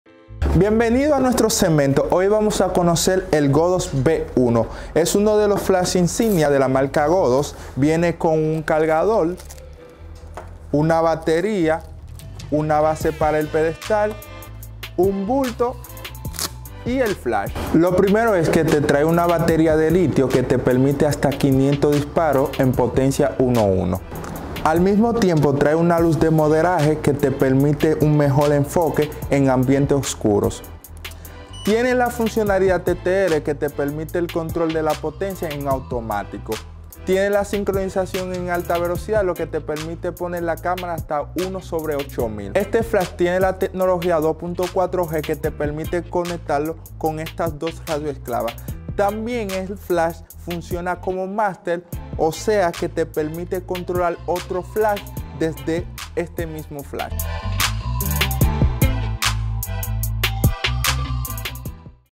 SEGMENTO CONOCE TU PRODUCTO ( T-1 #3 ) Godox V1 Flash
Características del flash Godox V1

Potencia del V1 : 76 Ws
Zoom de 28 a 105 mm. Con función auto flash y control manual
Cabeza giratoria de 0 - 330º
Inclinable de -7º a 120º de forma vertical
Potencia: 1/1 hasta 1/256
Duración del destello del flash de 1/300 a 1/20000 de segundo
Modos de uso: Automático TTL / Manual
Compensación de exposición del flash: +/-3 pasos en incrementos de 1/10.
Sincronización: HSS hasta 1/8000 de segundo, 1 y 2 cortinilla disponibles
Modo Multi-flash. Estroboscópico hasta 100 destellos a 199 Hz
Modos disponibles: Maestro, Esclavo, OFF
Grupos de control en esclavo: 4 (A,B,C,D)
Alcance en modo radio: 100 metros
Canales: 32 (1 a 32)
Número ID: 01-99
Luz de modelado. Se inicia al apretar el botón de profundidad de campo
Luz de ayuda al enfoque y rango efectivo:
Centro: 0.6-10 metros / 2.0-32.8 pies
Periferia: 0.6-5 metros / 2.0 a 16.4 pies
Luz de Modelado LED con un consumo de 2W
Temperatura de color: 3300ºK +/-200ºK
Alimentación del flash: Bateria de Li-Ion de 7.2V / 2600mAh de Panasonic
Tiempo de reciclado:Número de flashes por carga: Aprox: 480 disparos
90 segundos hasta apagado automático o sin recibir señal. Hasta 60 minutos en modo esclavo.
Medios de sincronizar el flash: Zapata (pins dedicados) y entrada Jack 2.5mm
Temperatura de color: 5600ºK +/-200K
Dimensiones de 76 x 96 x 197 mm